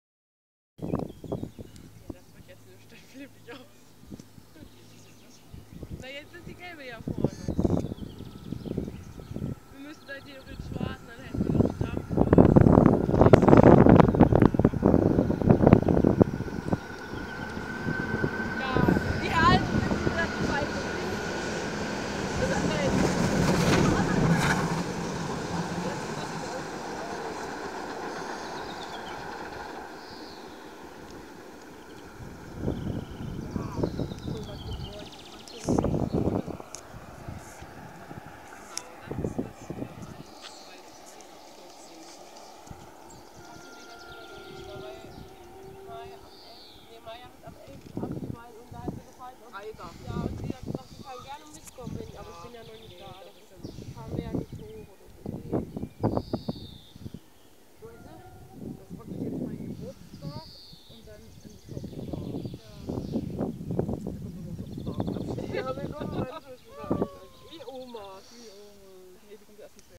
111 038 und 52 8079 unterwegs nach Bergen auf Rügen
111 038 (MTEG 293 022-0) und 52 8079 als Planzug unterwegs nach Bergn auf Rügen / Hier beim Durchfahren des Viadukts der Ehemailgen Kleinbahnstrecke Putbus - Altefähr am 15.6.14